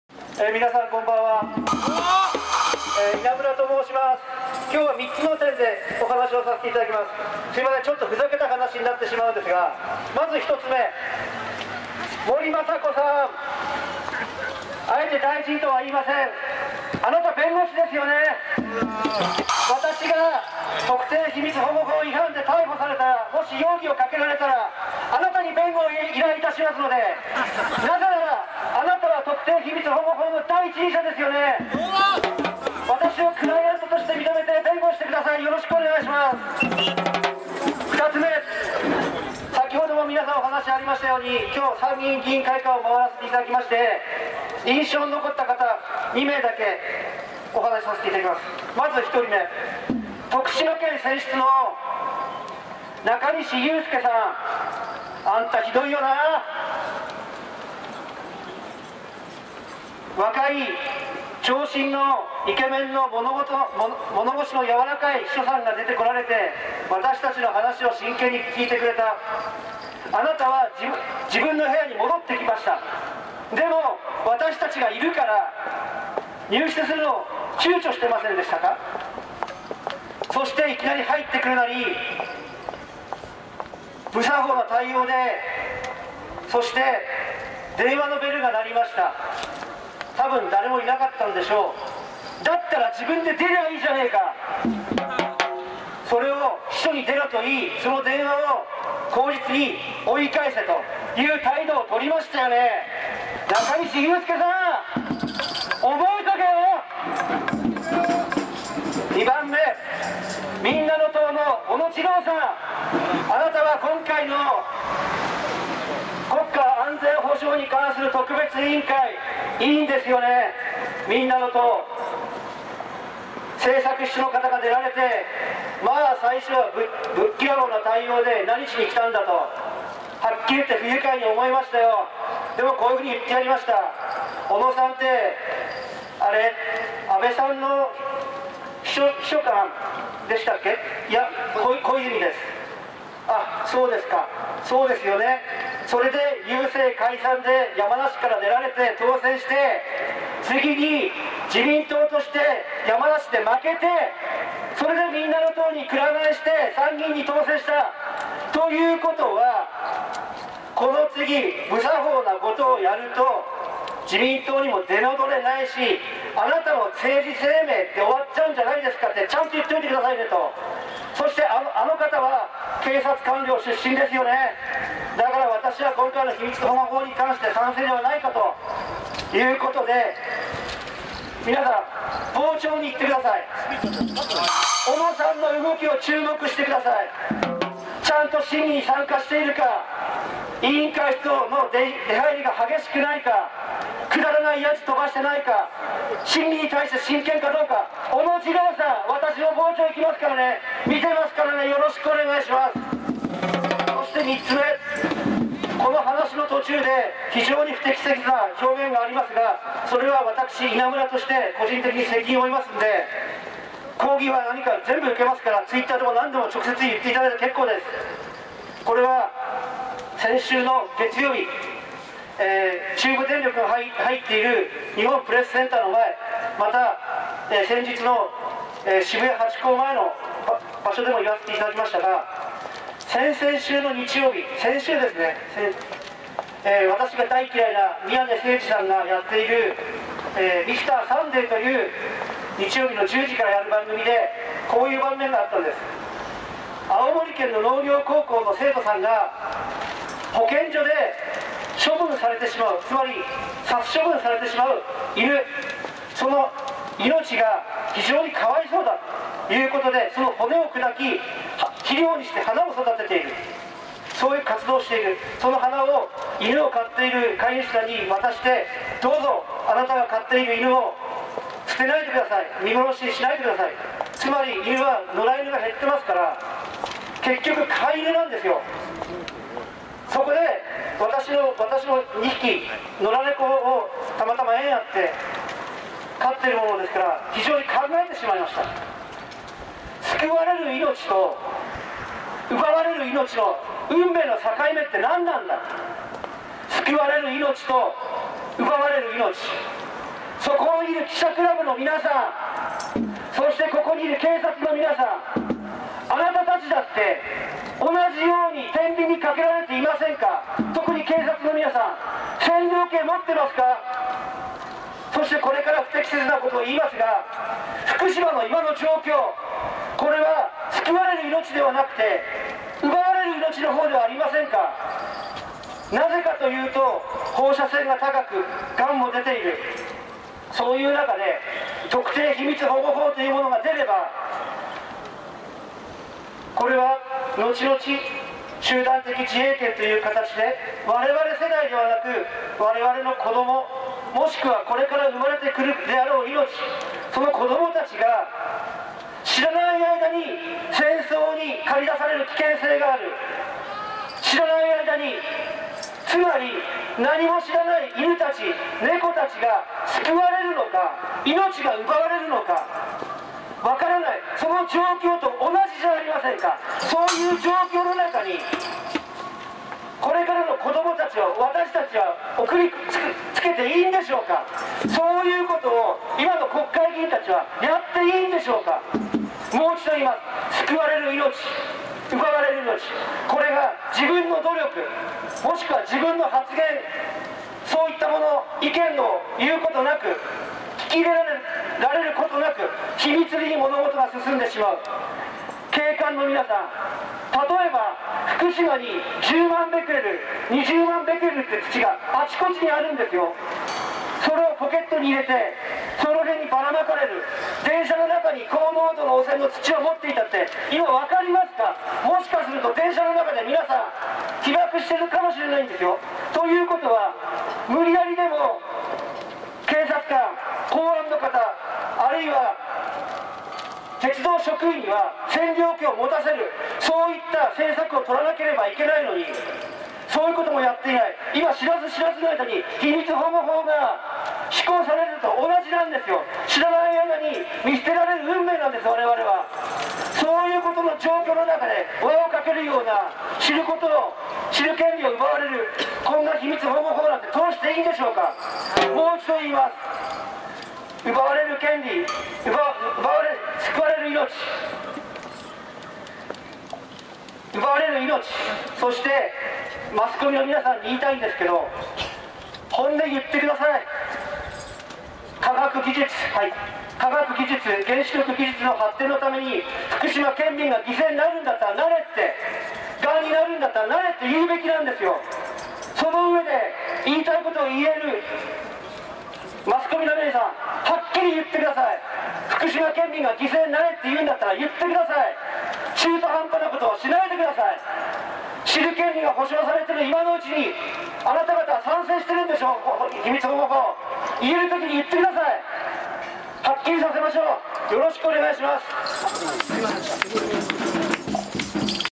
2013.11.27「STOP 特定秘密保護法案！官邸前アクション」中西祐介・小野次郎議員の失礼な対応《4/15》
『STOP 特定秘密保護法案！官邸前アクション』
★日付　１１月２７（水）
★時間　１８時半～２０時半
★場所　首相官邸前
★呼び掛け　火炎瓶テツと仲間たち